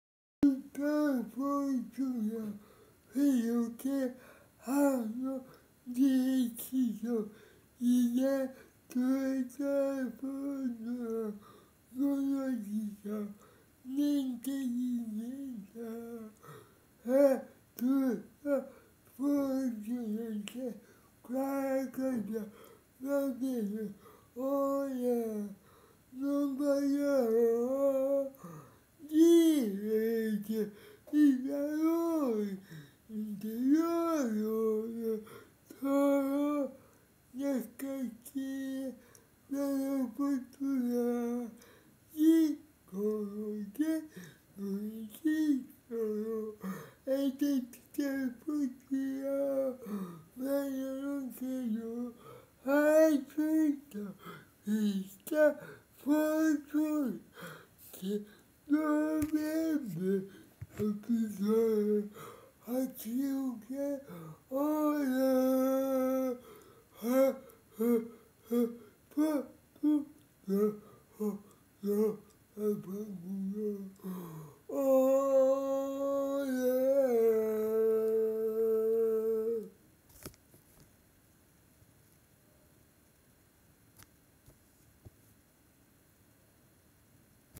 PARODIA MUTONIA TUTTA FORTUNA francobus100
DA 12 ANNI,  CON VOCE MISTICA, SENZA  MUSICA HO INVENTATO IL RAP HANDICAP.